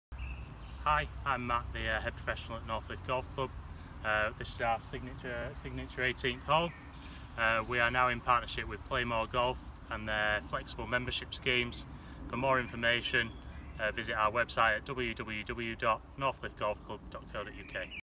PlayMore Golf Flexible Membership at Northcliffe Golf Club near Bradford
PlayMore Golf Flexible Membership now available at Northcliffe Golf Club near Bradford , for more information please go